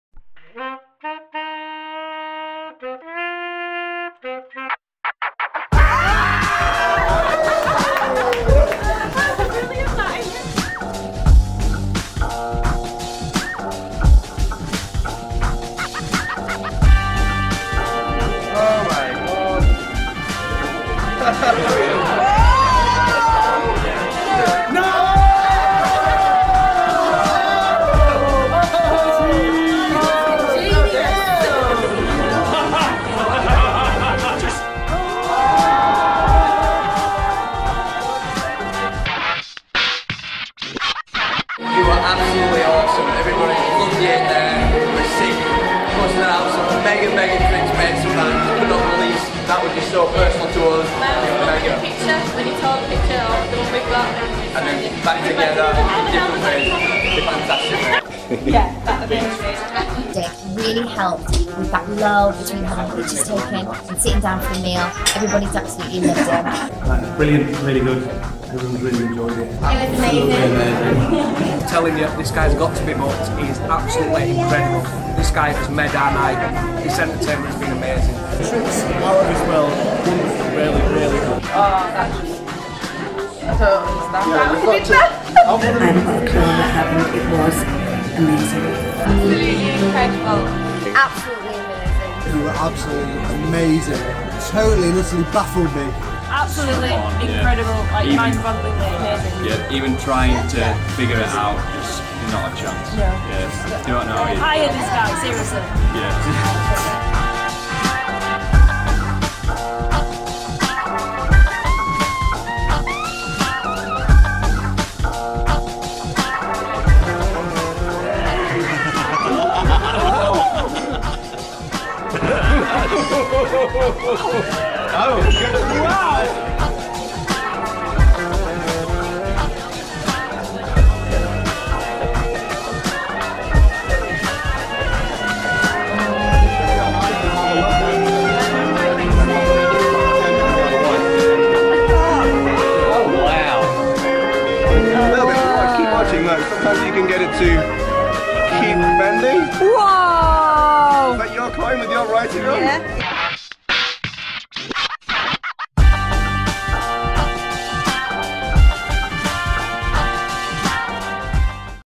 'Absolutely Amazing' Wedding Magician Oliver Parker 2017
Close Up Magician - Oliver Parker
Break the ice and blow their minds with fun, friendly interactive wedding entertainment!
Perfect to keep your guests busy during your afternoon reception and wedding photographs or to fill in the lull between the wedding breakfast meal and evening reception.
Treat your guests with something DIFFERENT to talk about!